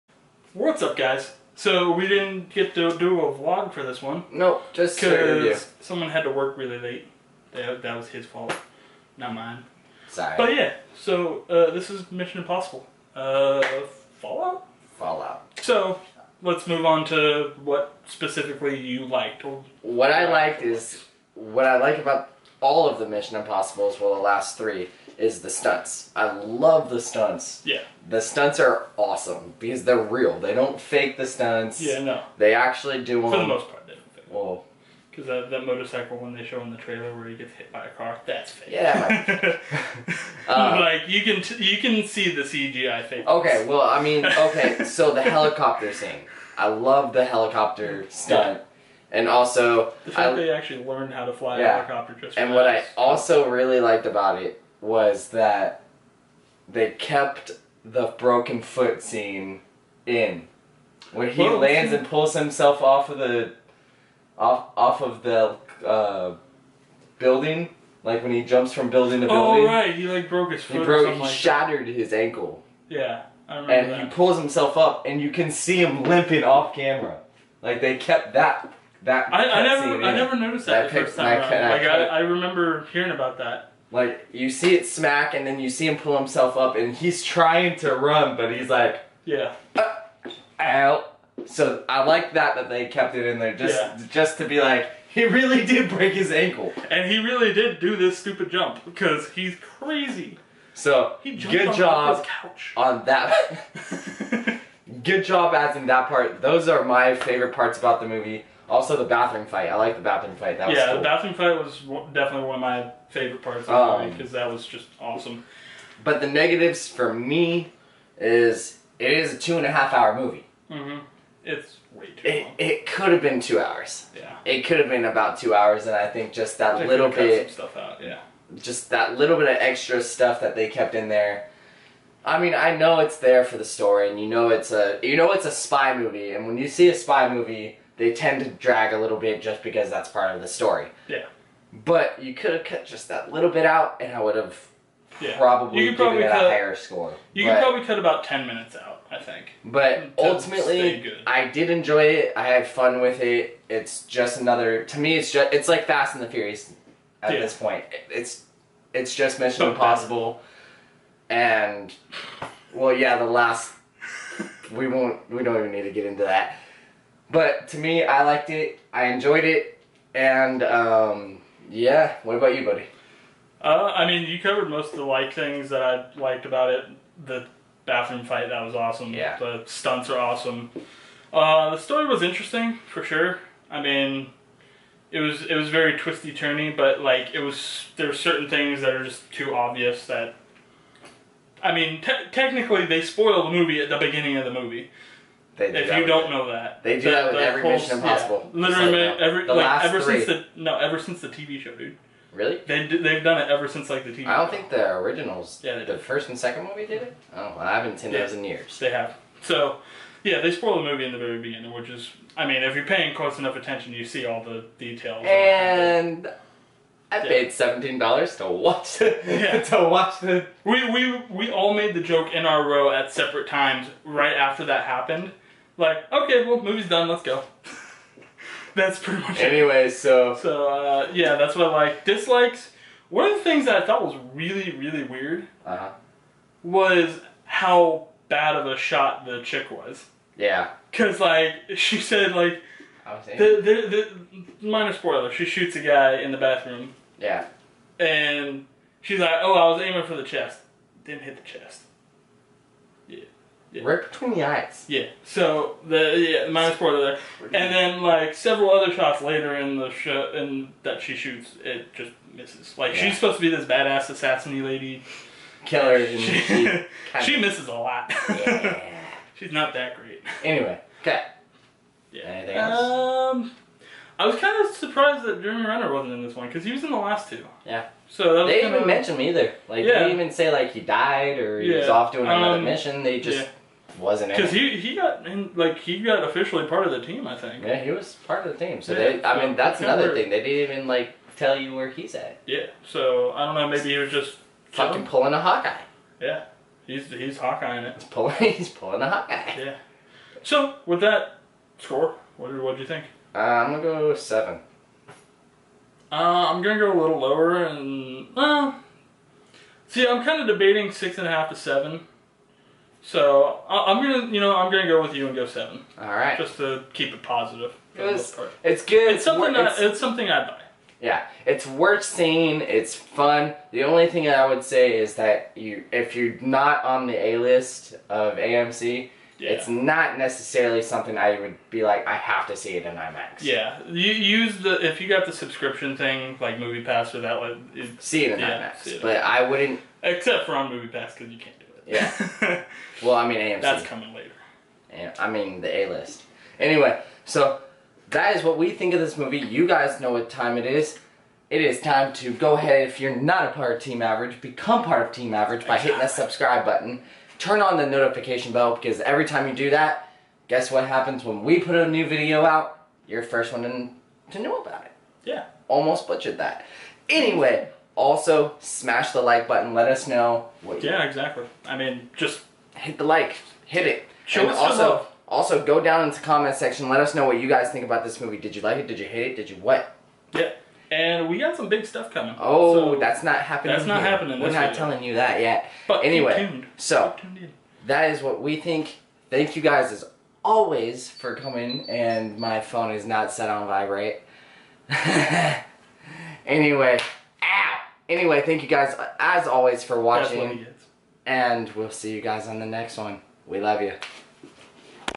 Mission Impossible: Fallout Movie Review
YAW reviews the newest summer blockbuster, Mission Impossible: Fallout.  Find out what we thought of the movie and see if it's worth accepting the mission!